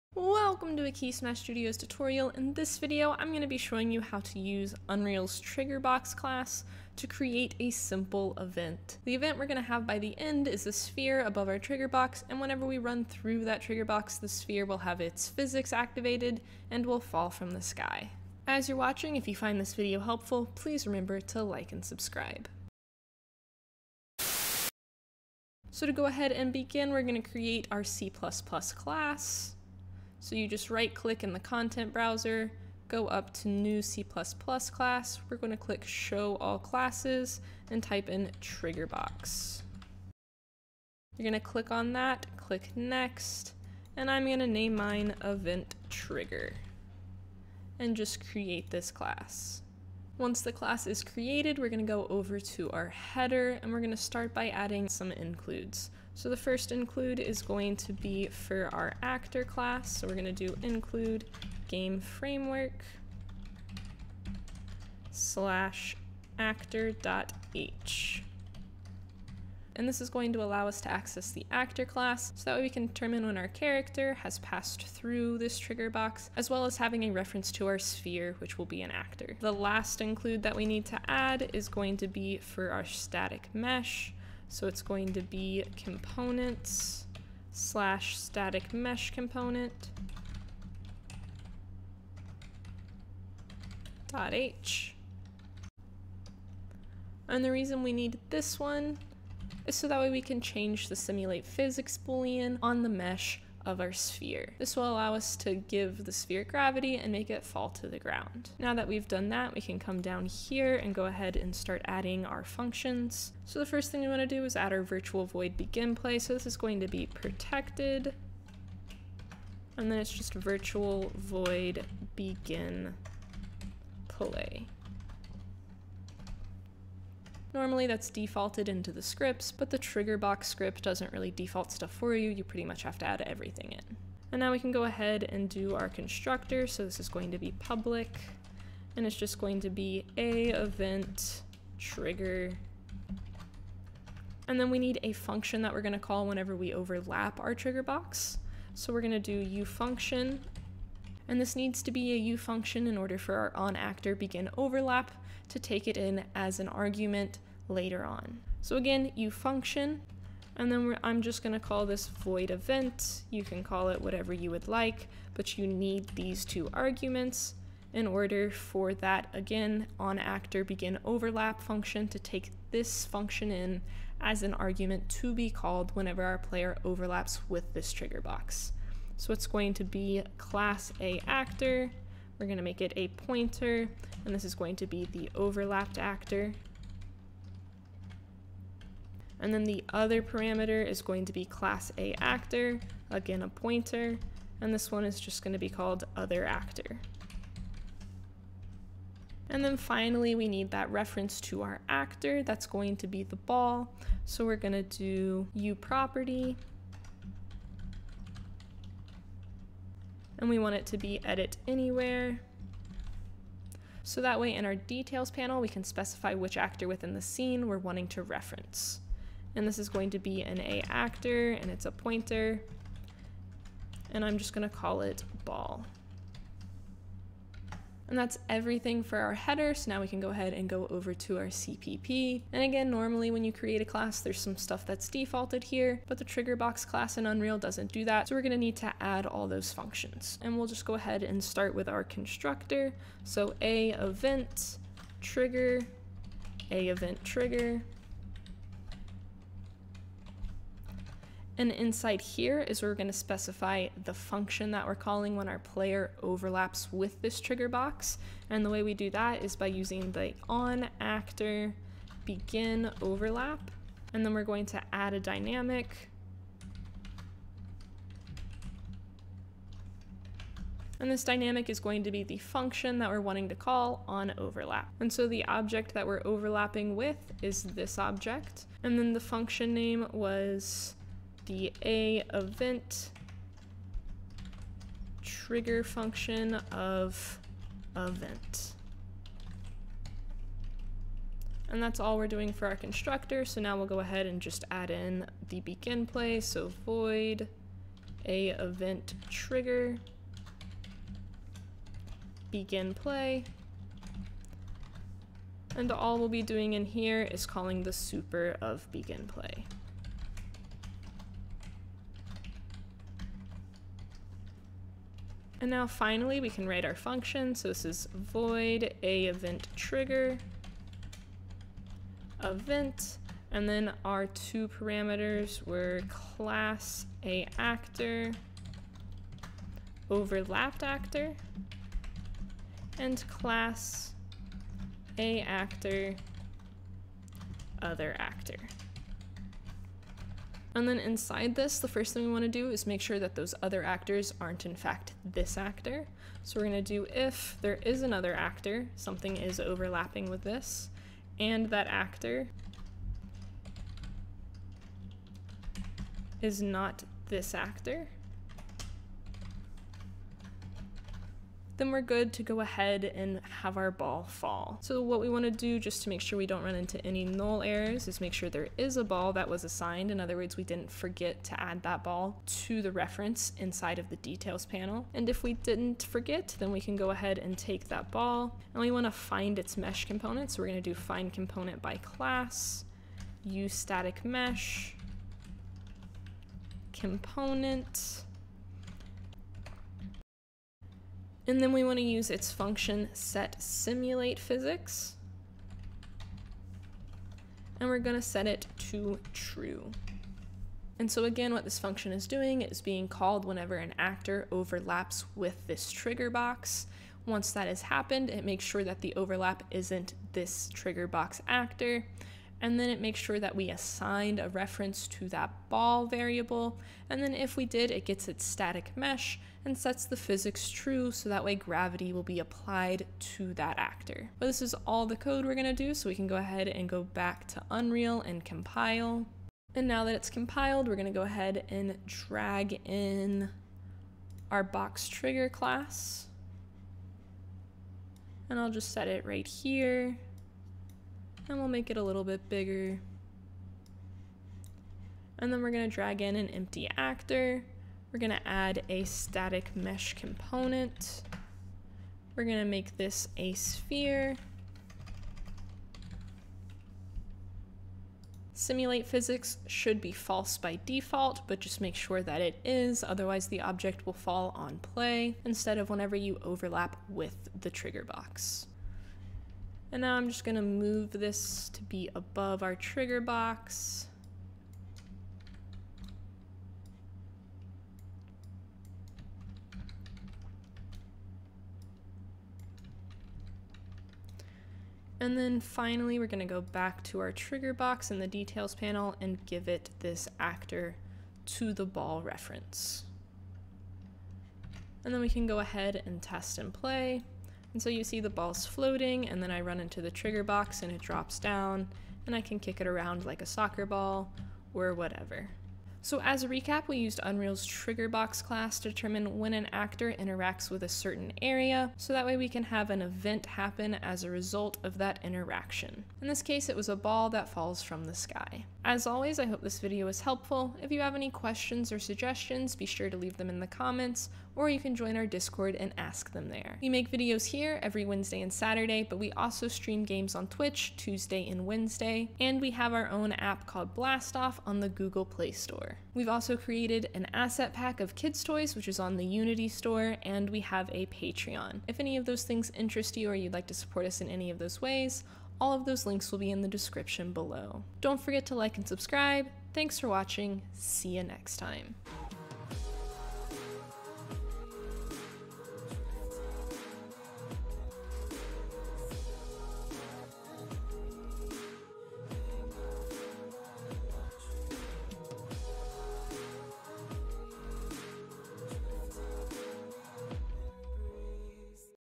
How to Trigger Events in C++ | Unreal Engine
Learn how to trigger events in C++.  This part covers how to use a trigger box, so that you can create events based off your players interactions.  Just follow along as Emily takes us step by step through an easy process to create events.  Do you have any questions?  Let us know down below!